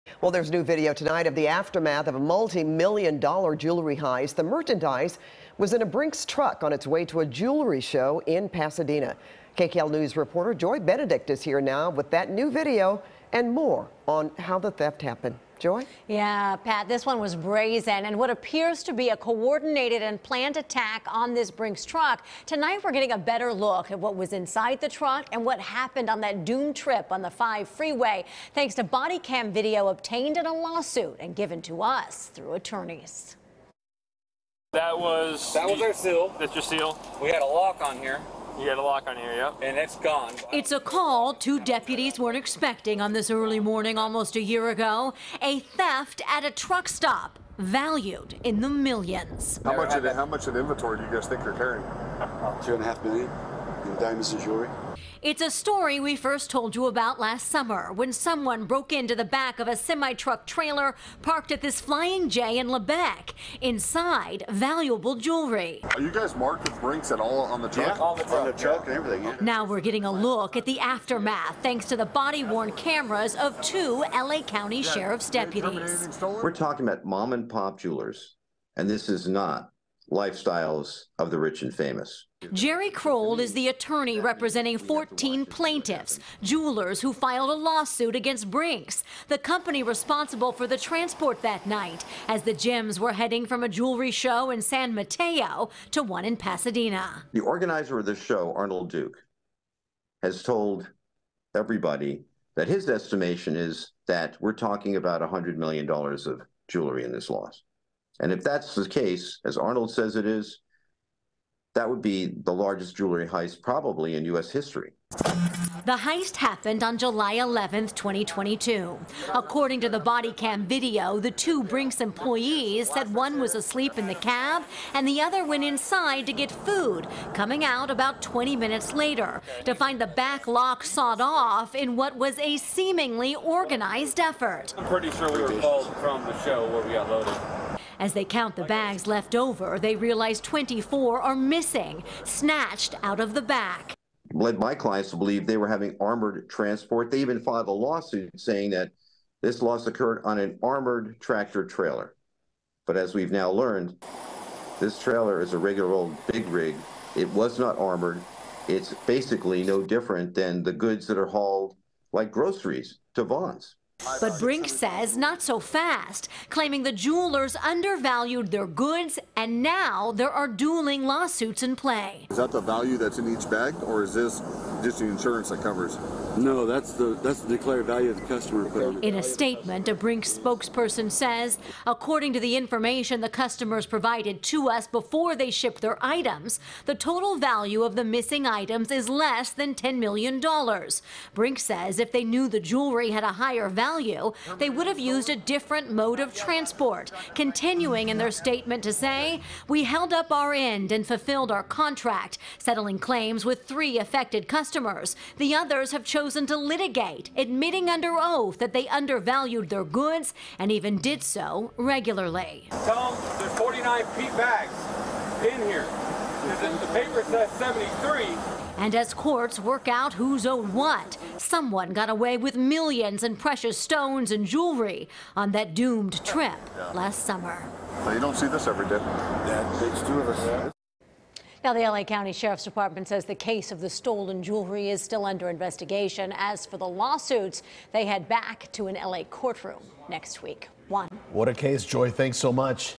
Bodycam footage shows events after millions of diamonds and jewelry are stolen from semi-truck
Bodycam footage has uncovered the moments following a multi-million dollar jewelry heist in Lebec. Almost a year ago, two security guards made a pit stop at a Flying J while transporting at least $10 million worth of jewelry from a show in San Mateo down to one in Pasadena. Tom Wait reports.